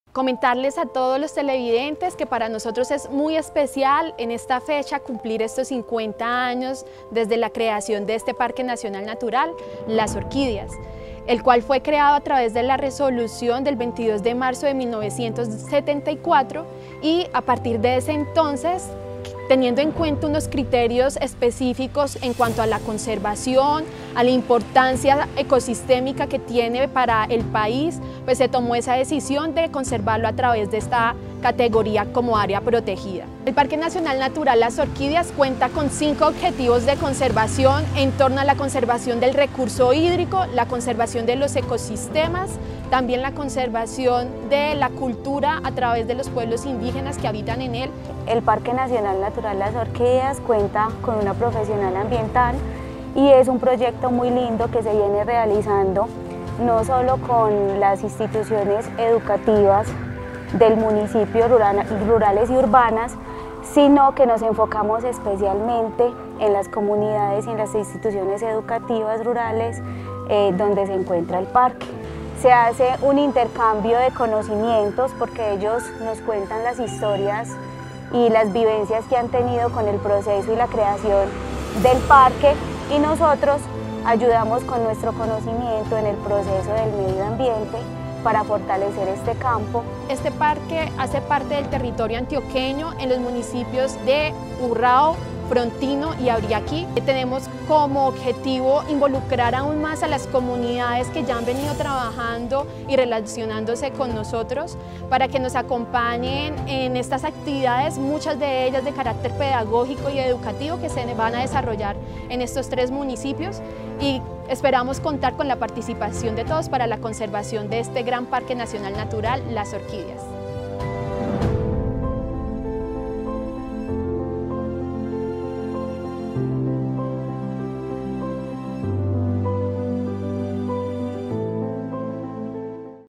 50 años Parque Natural Las Orquídeas
Veamos un poco del trabajo realizado en el lapso de los 50 años que esta semana cumplió el parque nacional natural las Orquídeas. Funcionarias de este nos hablan de su valiosa labor y desempeño en medio de la preservación de la naturaleza